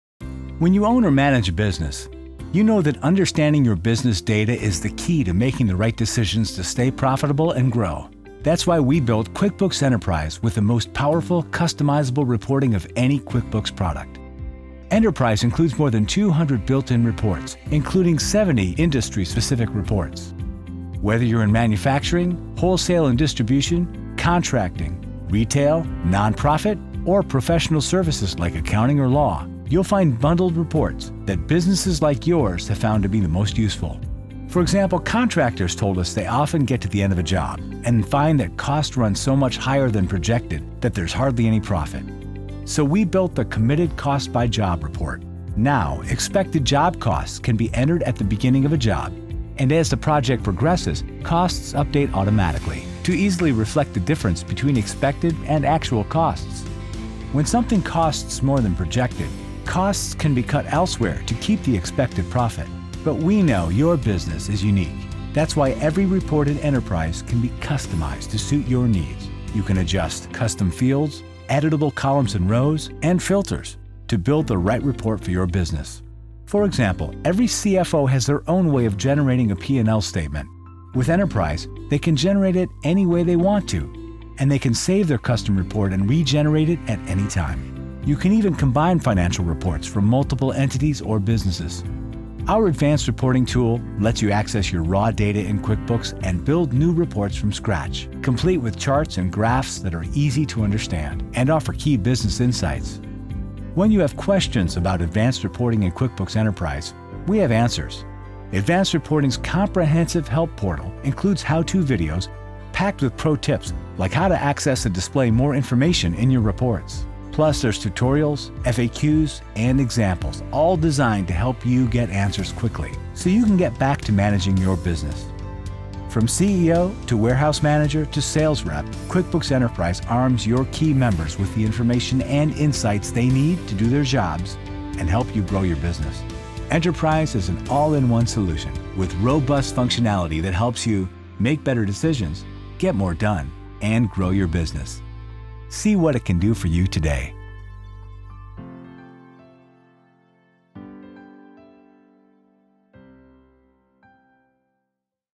Advanced Reporting | QuickBooks Enterprise
QuickBooks Enterprise was built with the most powerful, customizable reporting of any QuickBooks product. Pick from 200 built-in reports, including 70 reports that are industry-specific for Contractors, Manufacturers, Wholesalers & Distributors, Retailers, Nonprofits, Professional Services like accounting and law, and more. You can even build your own report or edit any existing report for your business’s unique needs. Plus, our comprehensive help portal will answer all your reporting questions. The powerful insights of Advanced Reporting will help you better understand your business data so you can make better business decisions.